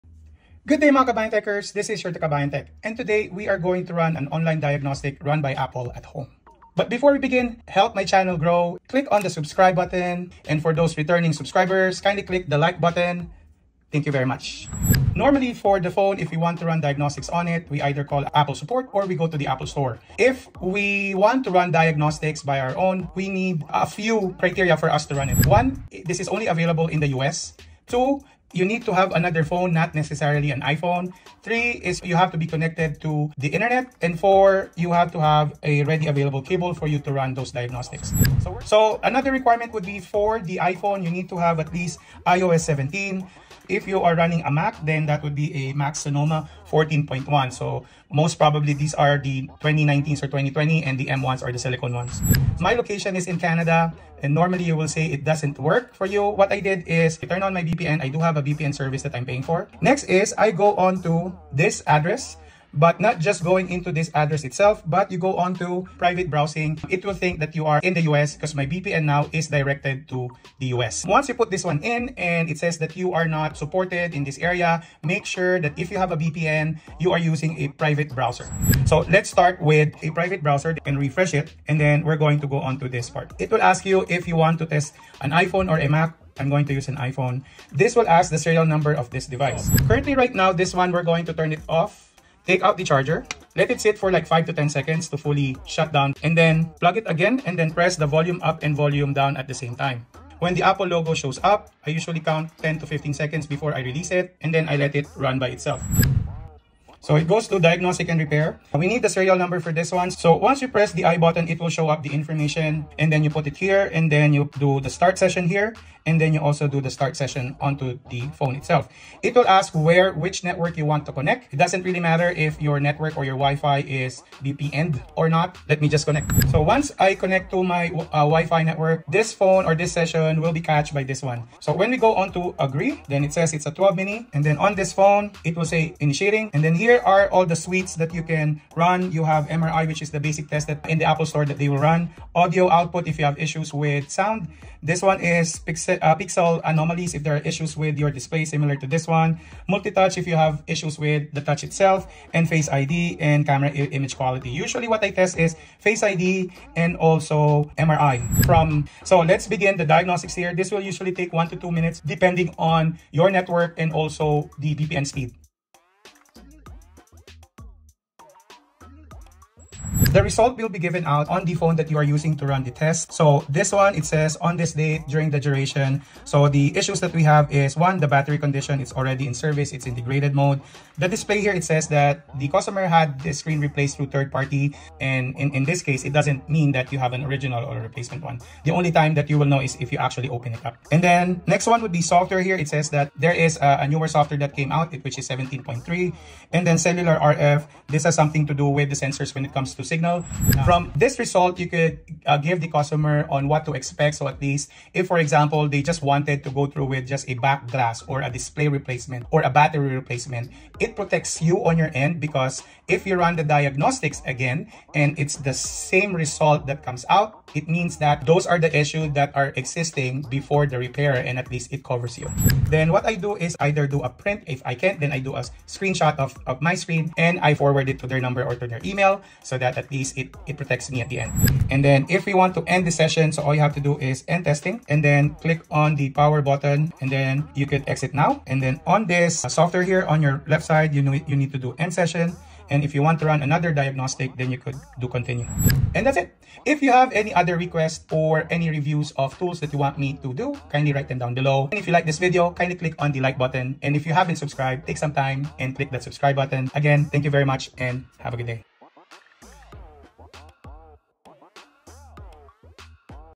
Running FREE iPhone Online Diagnostics via VPN
Sorry for the late post, currently out for the week. But as usual, I will try to post every Mondays & Fridays.

Alot of video popping up about running Apple's online diagnostics but unfortunately it is only offered in the US and running in a different country will show services not available for your country.  So I tried running it on VPN and was successful.  Stay tune for the Mac Online Diagnostics.